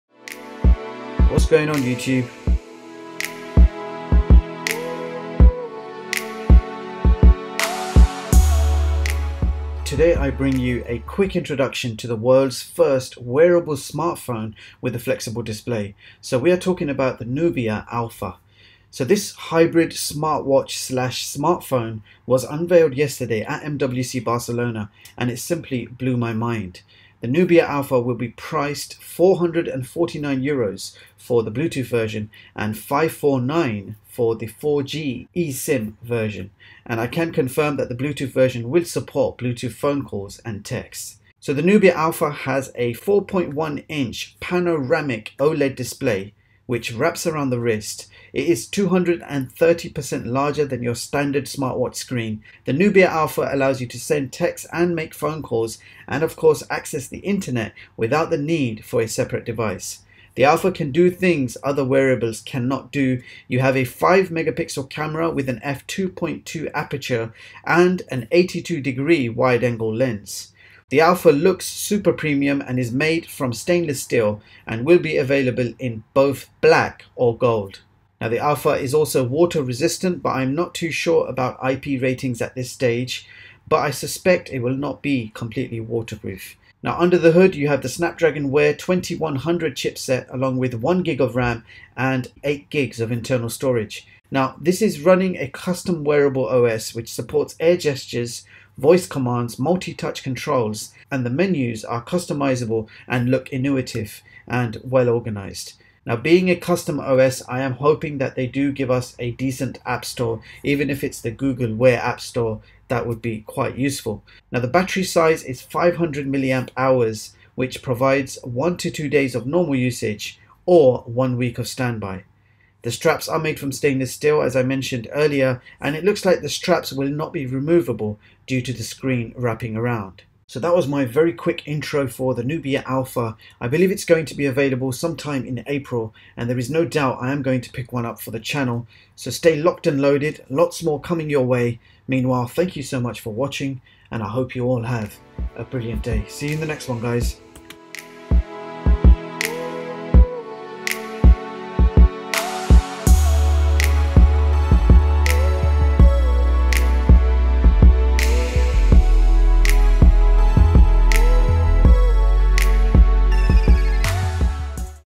World's First Wearable Smartphone / Smartwatch - Nubia Alpha
Nubia Alpha Wearable Smartphone / Smartwatch
NubiaAlpha is a flexible wearable smartphone with air gestures and 4-inch widescreen display.

Specs:
4.01" inch Flexible OLED Display
CPU: Snapdragon Wear 2100
1GB RAM
8GB Internal Storage
5MP Camera F/2.2 (82 degree lens)
Supports Bluetooth Calls/Texts
4G Version uses eSIM
Custom OS
500mAh Battery (1-2 Days)
Non-Removal Straps
Water Resistant (Not Waterproof)